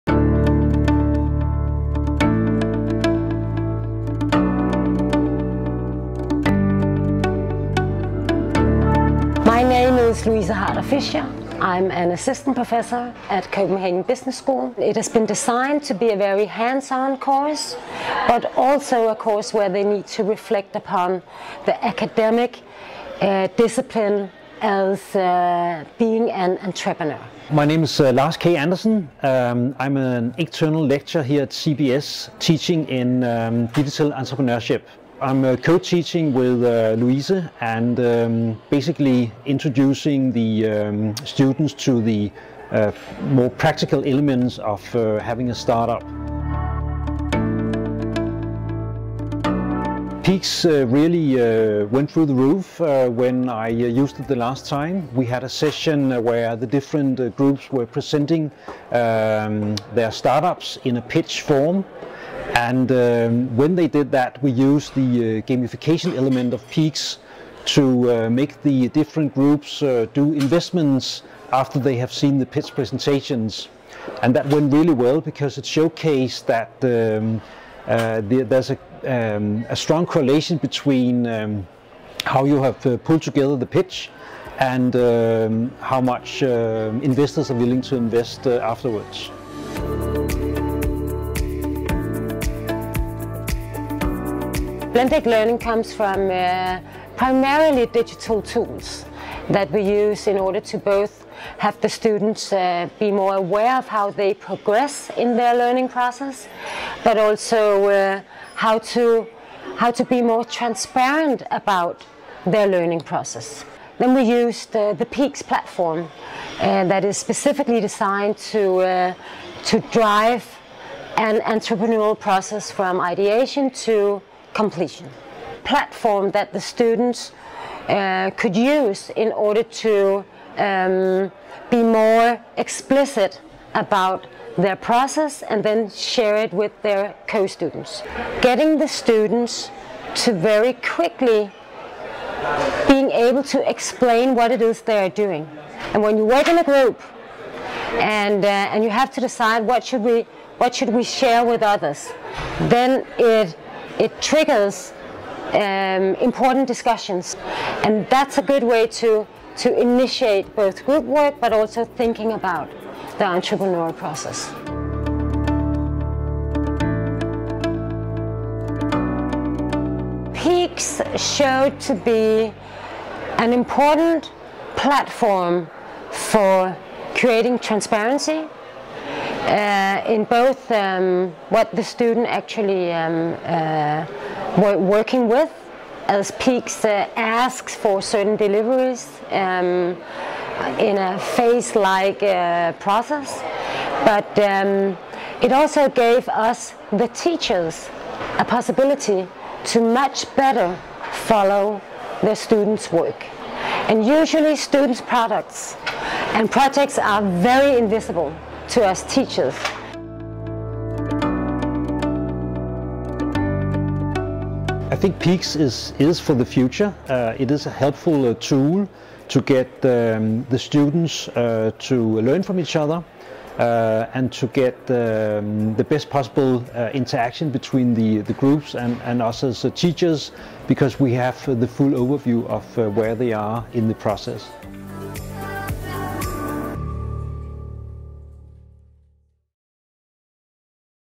Peaqs used in Copenhagen Business School - Digital Entrepreneurship Msc
During the fall semester of 2019 coordinator and lecturers Louise Harder Fischer and Lars K. Andersson implemented Peaqs in the Digital Entrepreneurship is a masters elective at Copenhagen Business School.
Here are their experiences and impressions with the Peaqs and the way it helps enabled them with adding blended learning to their teaching activities.